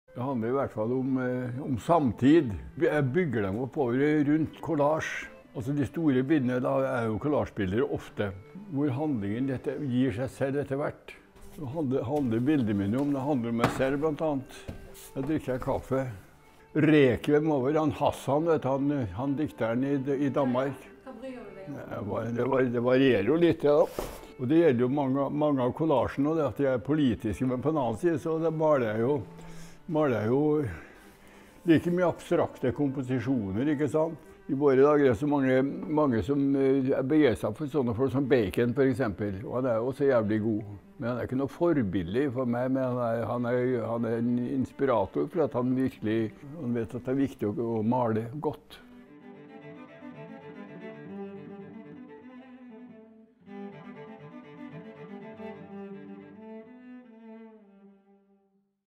#200 KUNSTNER: Håkon Bleken er en av Norges største kunstnere
Bli med inn i studio til Håkon Bleken, en av Norges desidert største nålevende kunstnere. Vi snakker om nye prosjekter, teknikker, gatekunst, Korona og savnet etter å reise. Se full episode 200 med Bleken på Kunzt.no